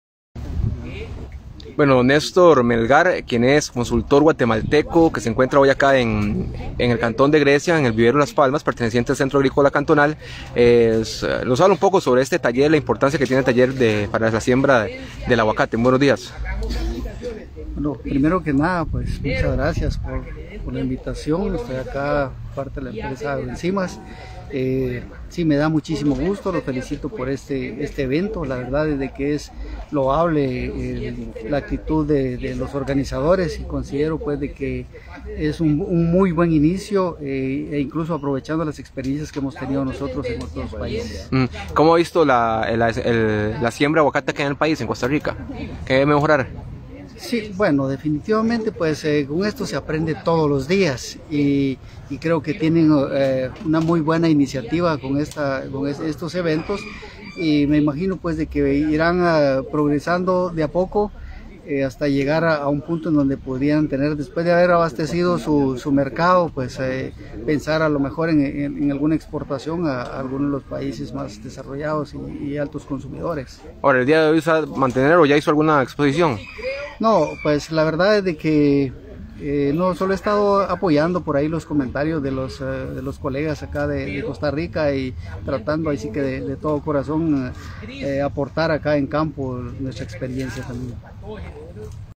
Nestor Melgar, consultor de aguacate invitado al taller que se realiza en el vivero Las Palmas.
Nestor Melgar, consultor de aguacate invitado al taller que se realiza en el vivero Las Palmas del Centro Agrícola Cantonal.